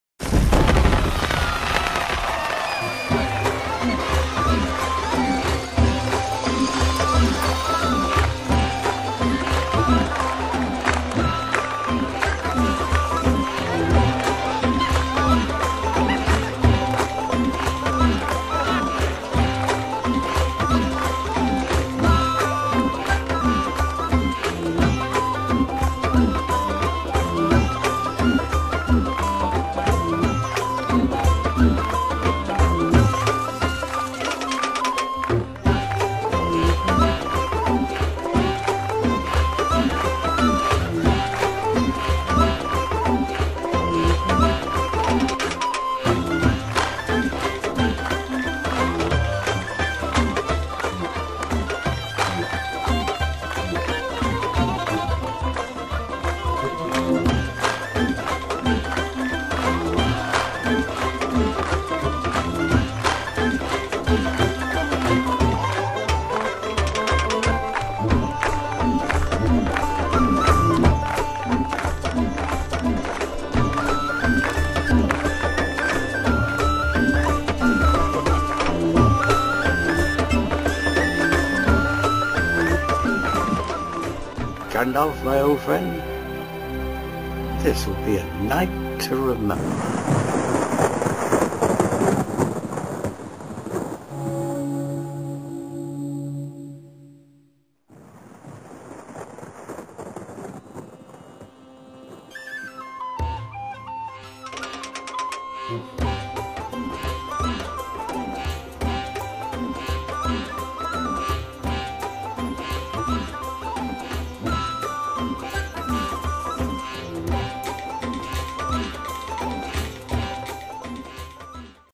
A Long-Expected Party T.A. 2008
Welcome to the Shire! Several days of games, song, food, drink, fellowship & adventure ending in a night to remember.